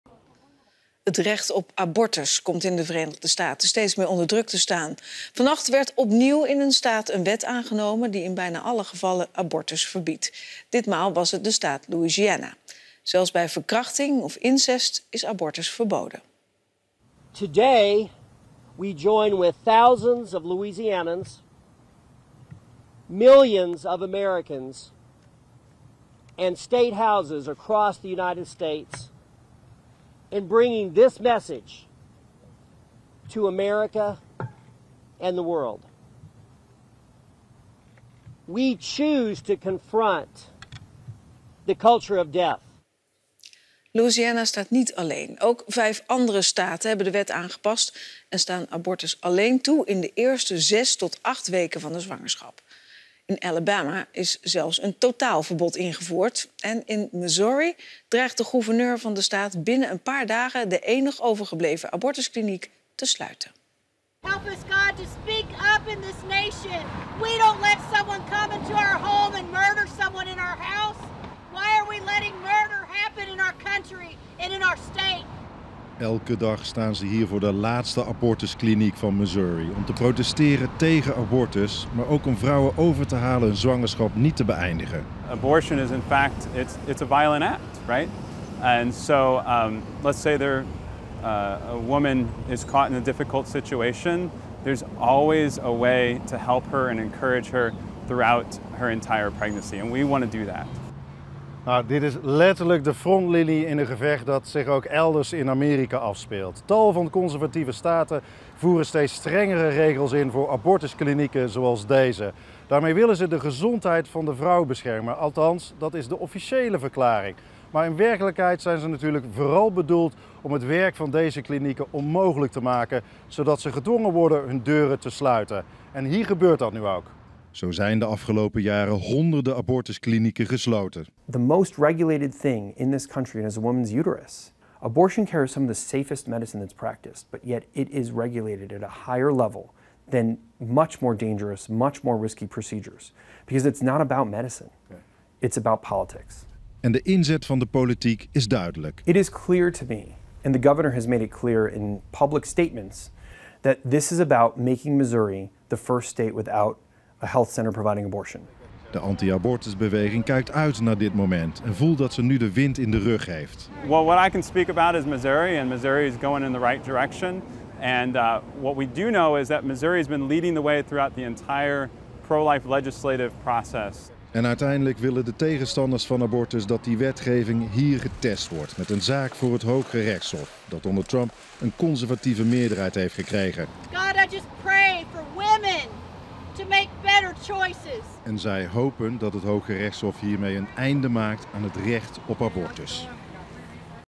De anti-abortusbeweging VS krijgt vleugels
Met hernieuwde energie hebben tal van conservatieve staten in Amerika het recht op abortus ingeperkt. Ze hopen hiermee een nieuwe uitspraak van het Hooggerechtshof te ontlokken , die onder Trump een conservatieve meerderheid heeft gekregen. Missouri gaat nog een stap verder. Daar dreigt de gouverneur de laatste abortuskliniek van de staat te sluiten. Verslag uit St Louis.